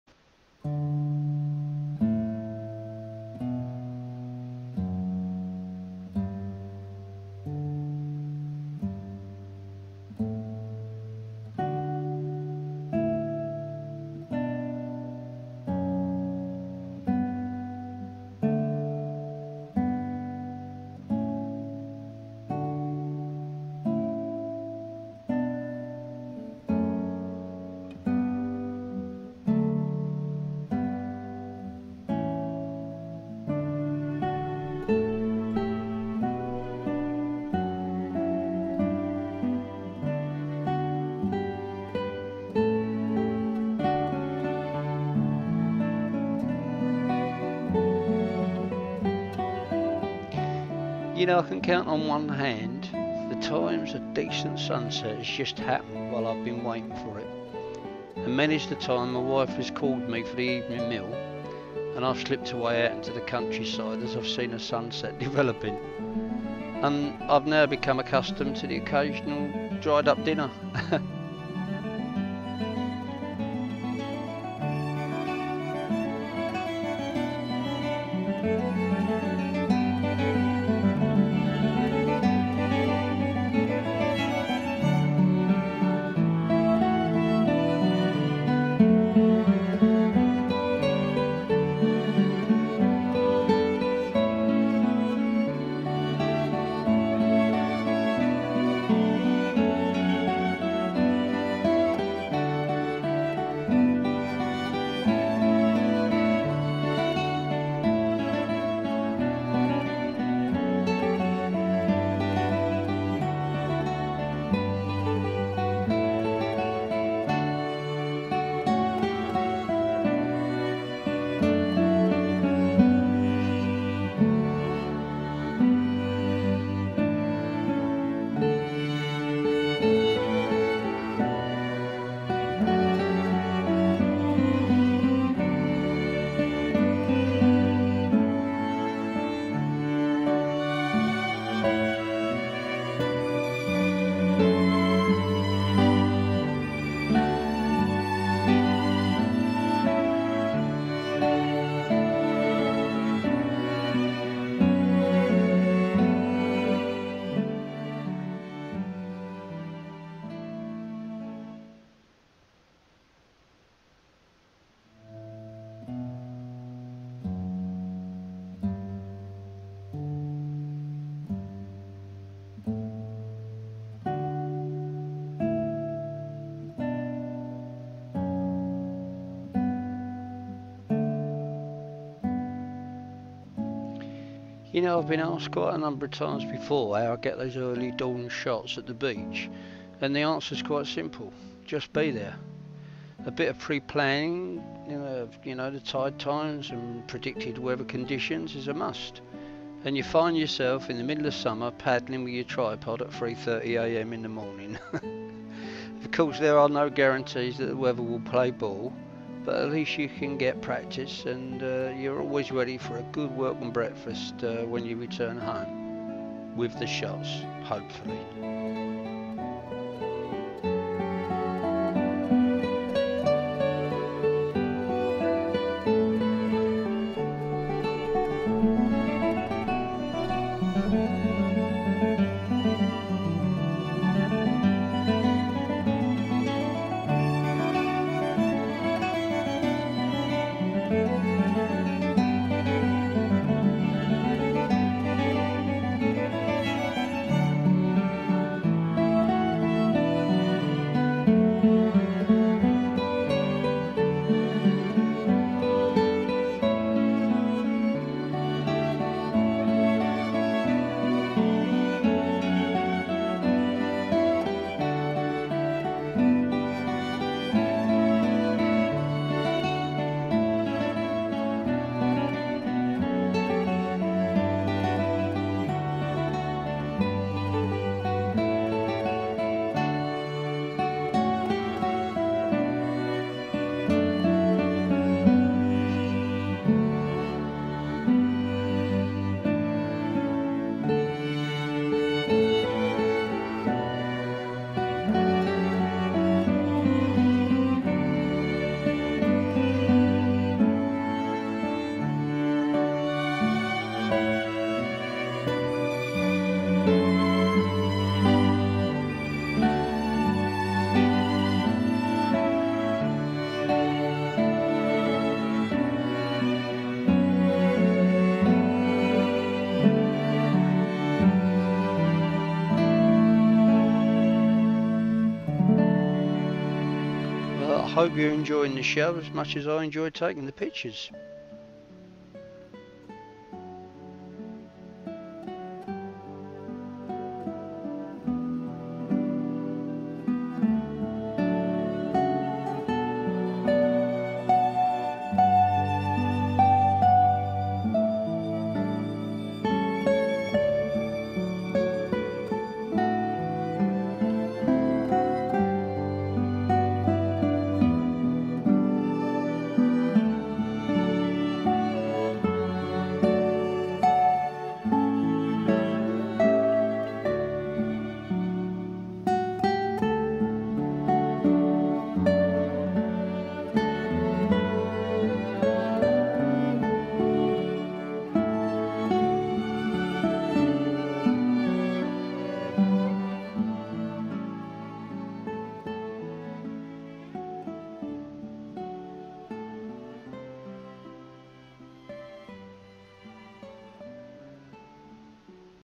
The Beginning and End of the Day
Sunrise and sunset around North Norfolk